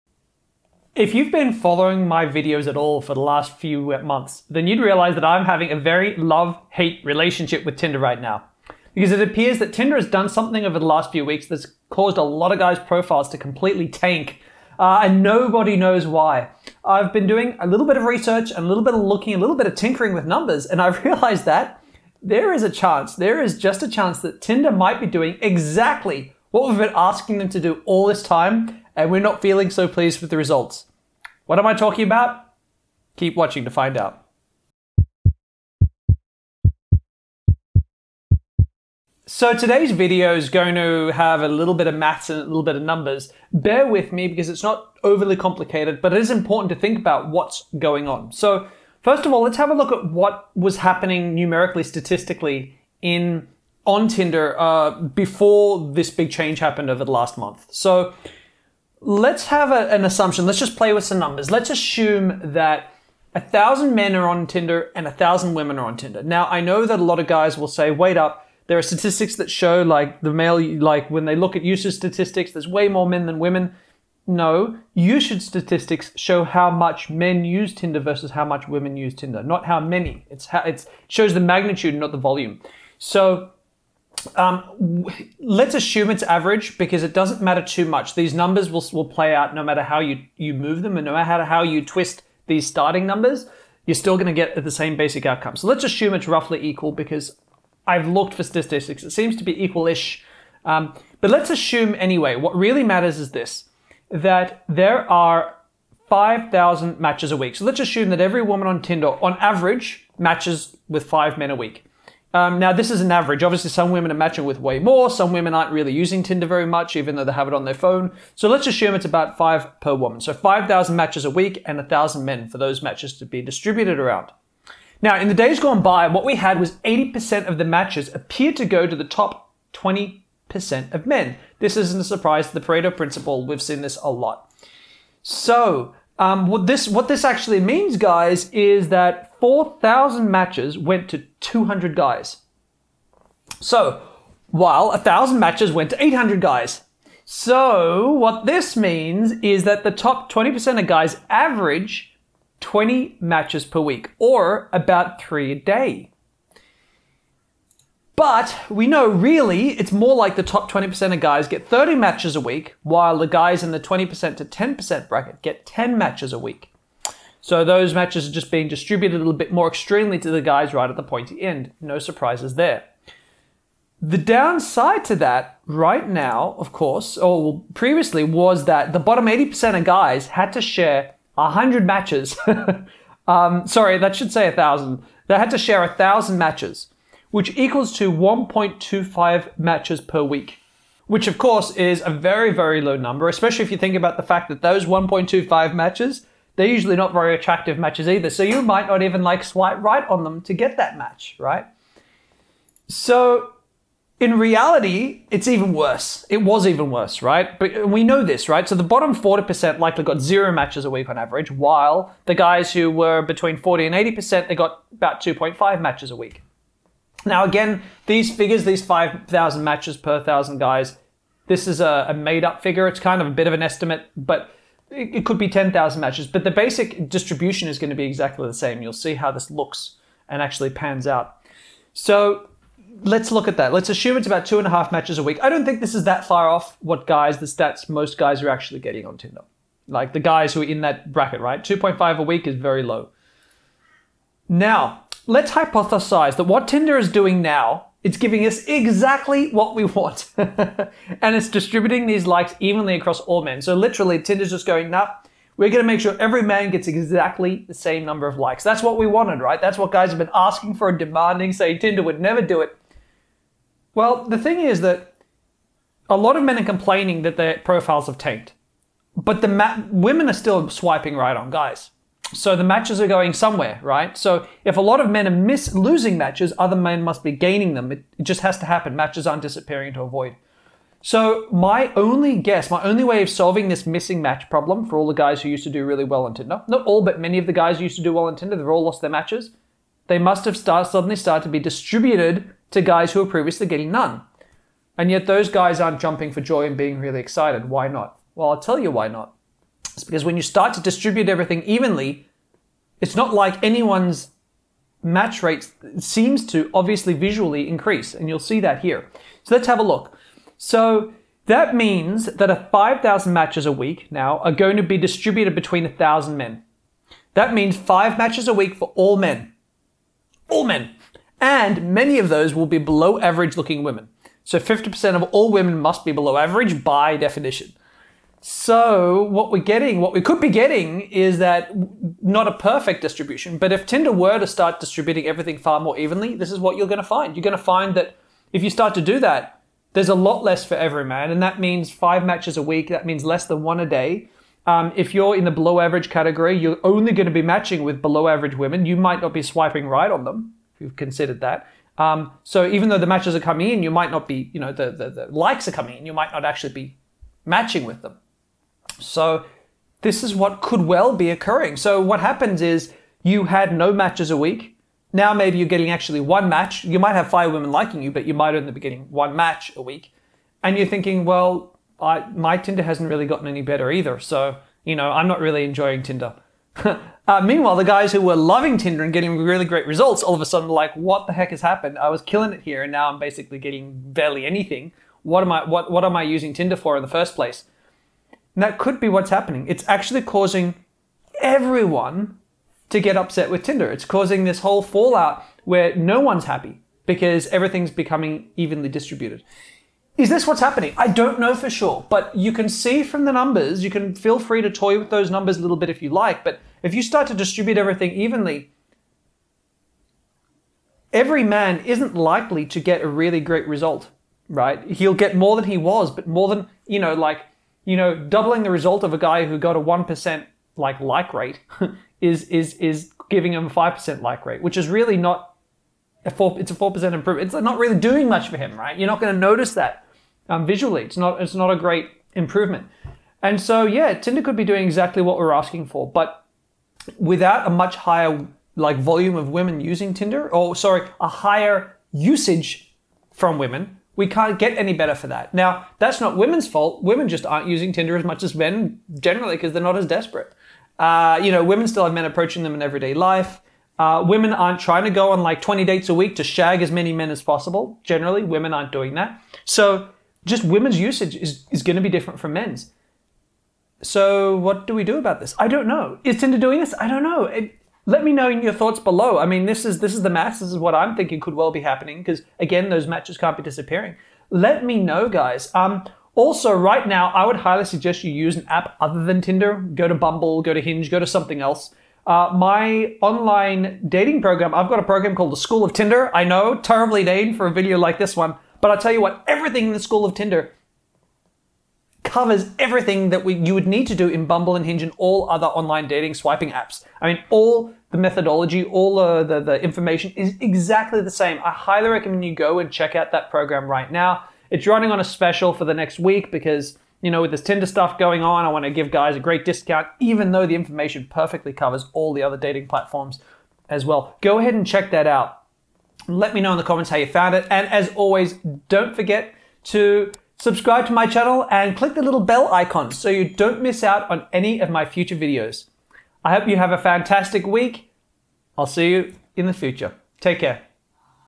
Tinder Did EXACTLY What We Asked - And We're MAD About It...
Has Tinder really gone and done exactly what we've asked them to do, and now we're turning around and being mad at them?

It's not secret over the last few weeks that a lot of guys have noticed their Tinder match rates have begun to seriously tank. Everyone (including myself) has been getting really annoyed with tinder.

But I've begun to tinker with the numbers that Tinder must be faced with, and I'm starting to realise something - IF Tinder went ahead and did EXACTLY what we asked them to do - distribute match rates and likes evenly across all men, that the results we'd expect is a pretty close match to what many men are experiencing right now!

School Of Attraction